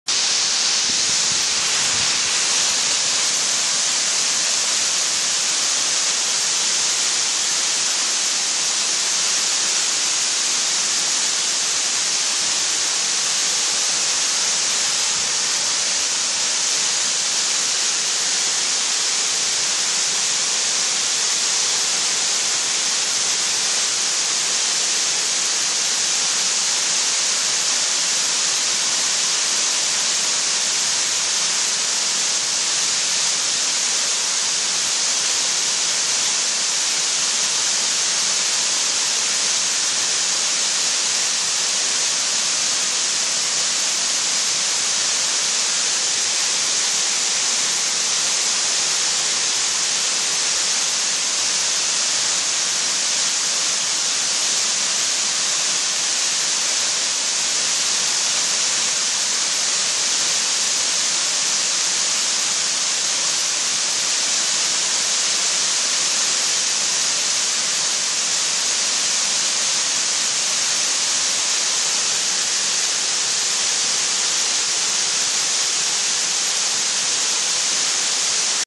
Pohnpei waterfall, Micronesia.
Kepirohi waterfall on Pohnpei island, Micronesia.